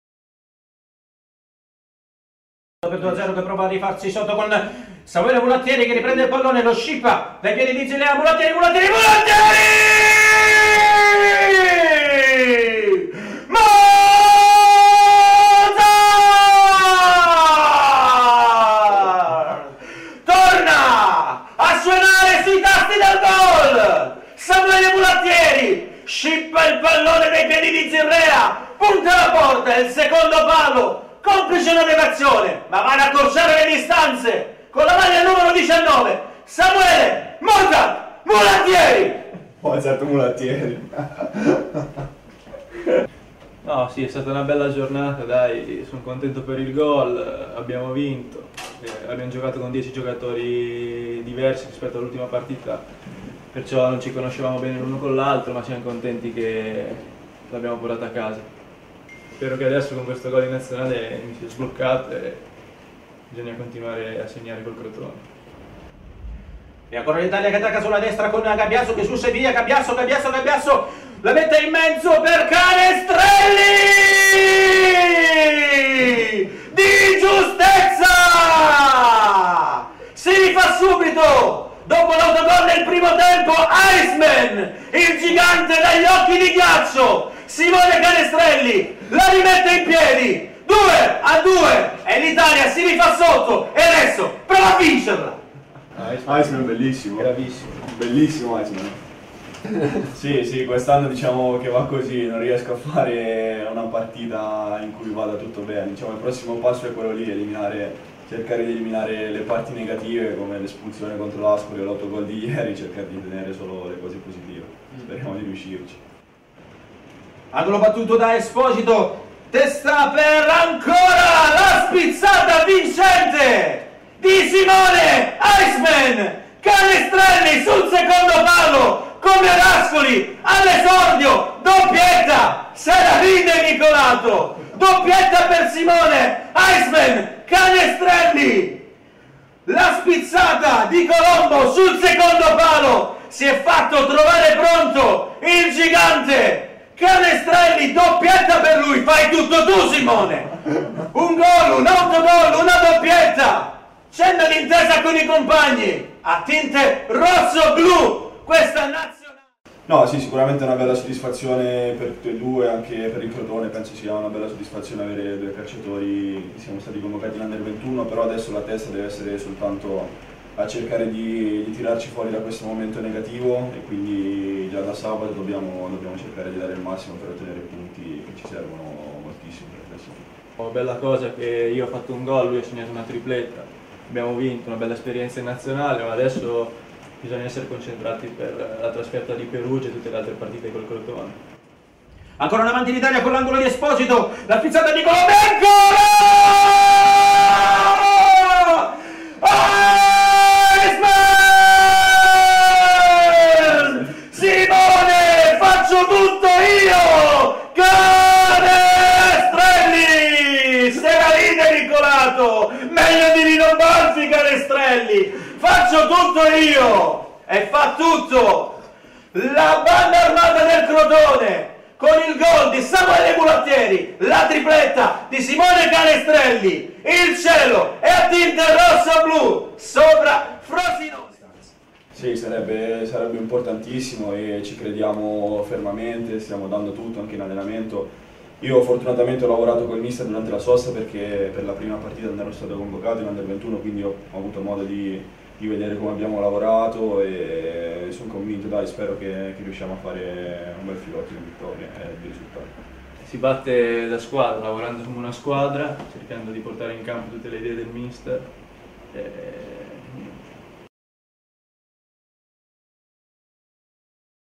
Canestrelli e Mulattieri verso Perugia dopo la grande serata con l'U21
🎙 Abbiamo fatto commentare la super prestazione degli azzurrini rossoblù al radiocronista di FCRadio con due spettatori d'eccezione... Ecco il risultato! ⚽️⚽️⚽️⚽️🔥